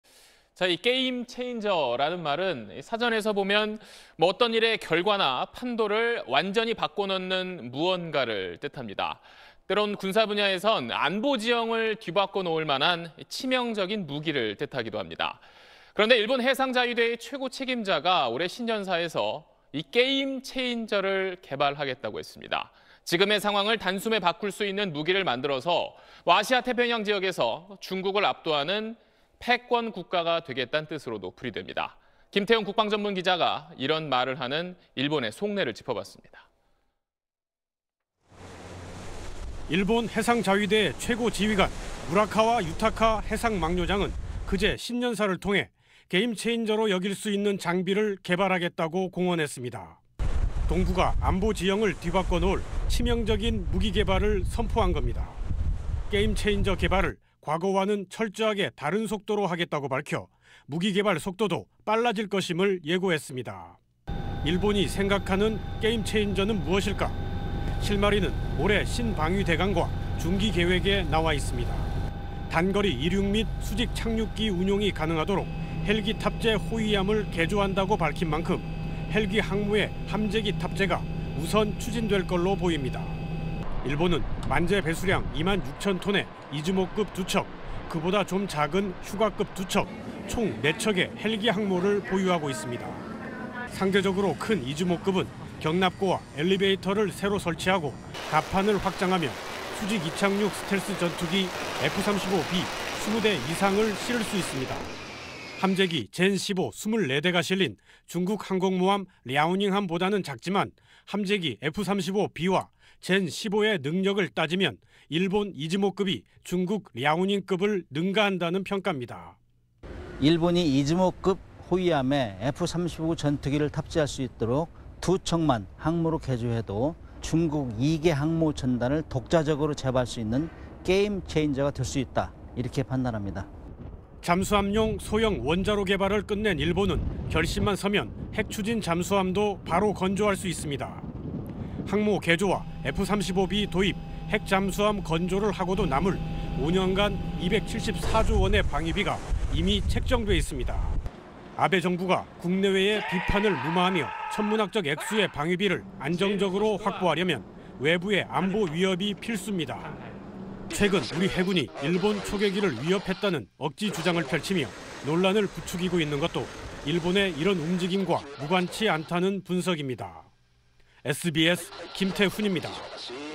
일본, 날카로운 발톱 드러냈다…'게임 체인저' 개발 선포 / SBS
'게임 체인저'라는 말은 사전에서 보면 어떤 일의 결과나 판도를 완전히 바꿔 놓는 무언가를 뜻합니다. 때로는 군사 분야에서는 안보지형을 뒤바꿔놓을 만한 치명적인 무기를 뜻하기도 합니다. 그런데 일본 해상 자위대의 최고 책임자가 올해 신년사에서 이 게임 체인저를 개발하겠다고 했습니다. 지금의 상황을 단숨에 바꿀 수 있는 무기를 만들어서 아시아 태평양 지역에서 중국을 압도하는 패권 국가가 되겠다는 뜻으로도 풀이됩니다.
김태훈 국방 전문 기자가 이런 말을 하는 일본의 속내를 짚어봤습니다.